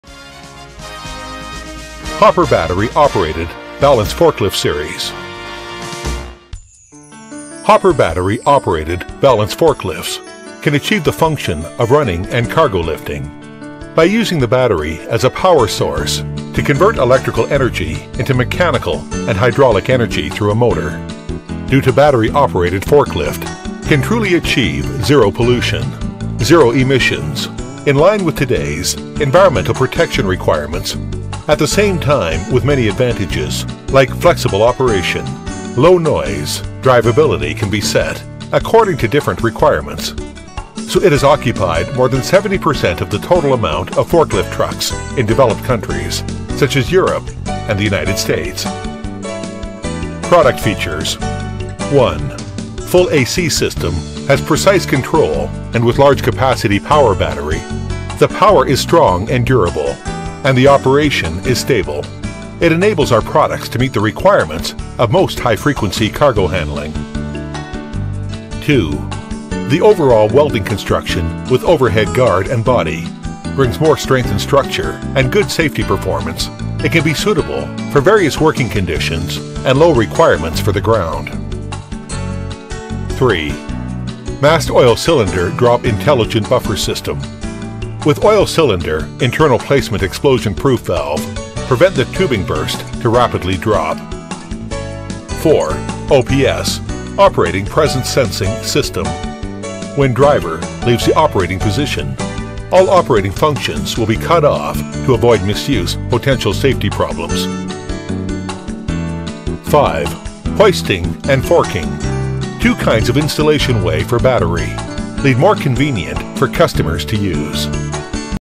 electric forklift series
Marshell Electric Forklift is an ideal four-wheel electric lift truck solution for most indoor and warehouse applications. The Electric Forklift is capable of moving most any standard load while maintaining the ability to fully operate in small spaces.